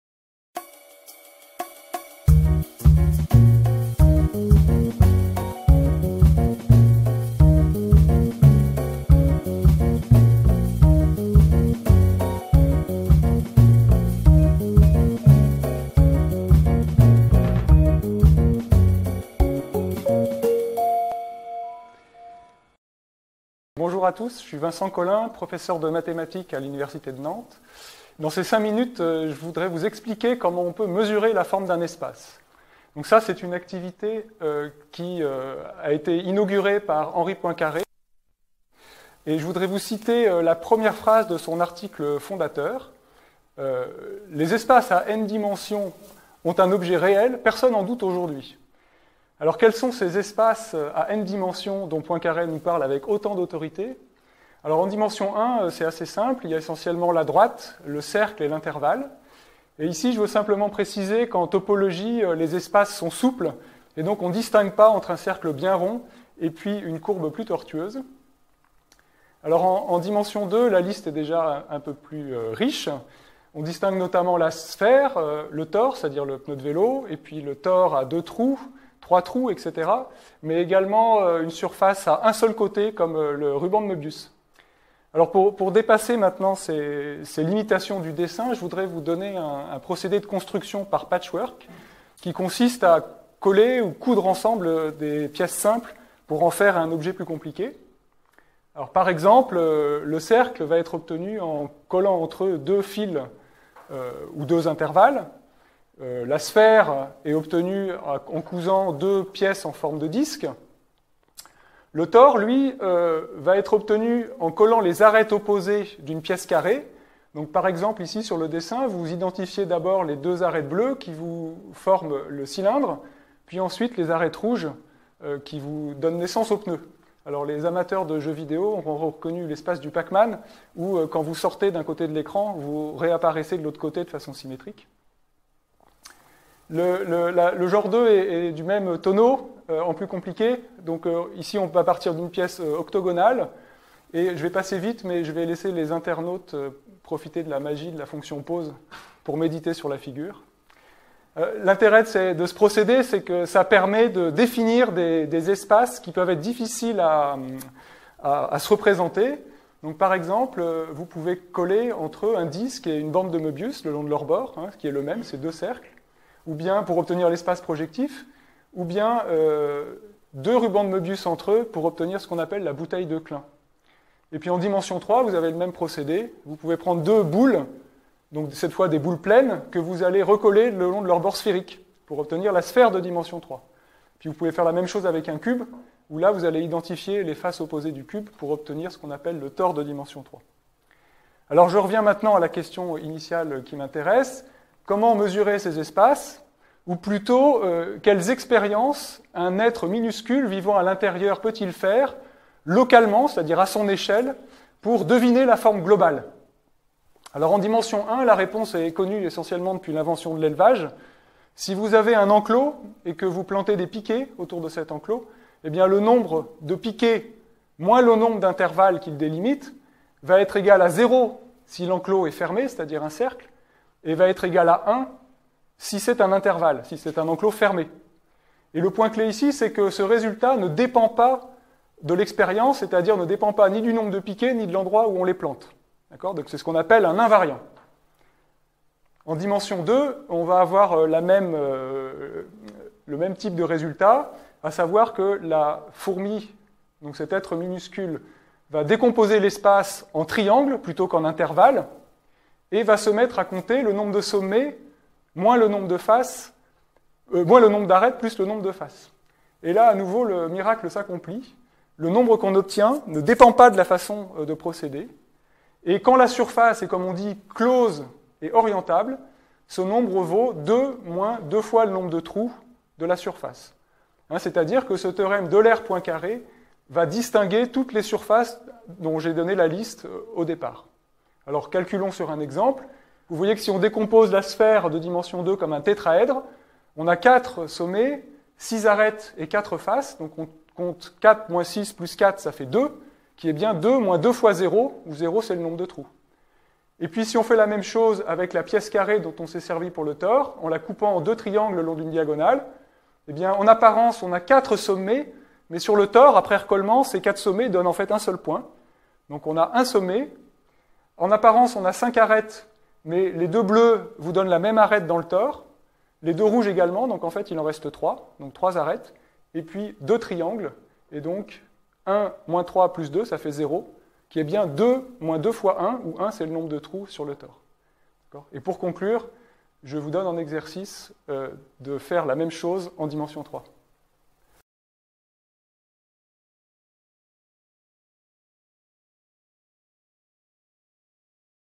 Vincent Colin - Comment mesurer la forme d'un espace ?
Comment construire des espaces exotiques aux propriétés surprenantes et comment, par des expériences locales, en deviner la forme ? On se laissera guider par Henri Poincaré.